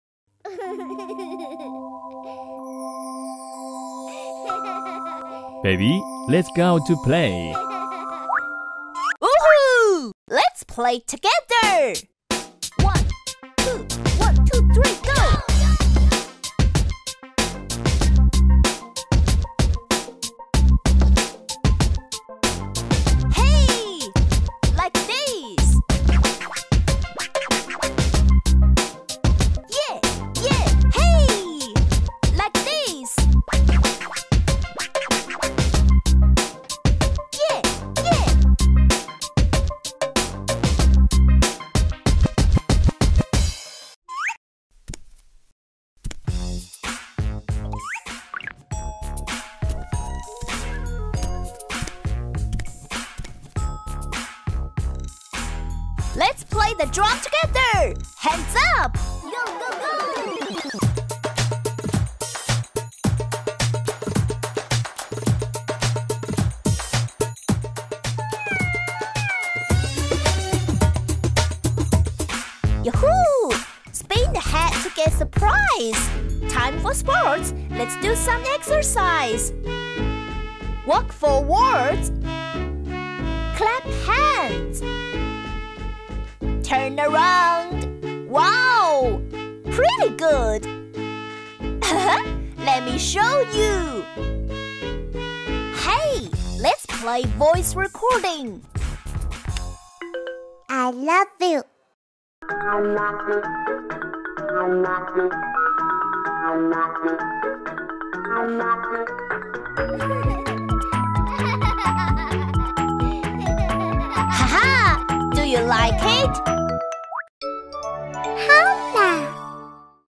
Музыкальная игрушка Hola Барабан (6107)
От 1-го года

       Музыкальная игрушка Hola Барабан забавная игрушка, которая позволит устраивать зажигательные танцы и проигрывать забавные мелодии. Барабан может записывать слова, а затем воспроизводить их. Предусмотрены кнопки, которые активируют голоса животных.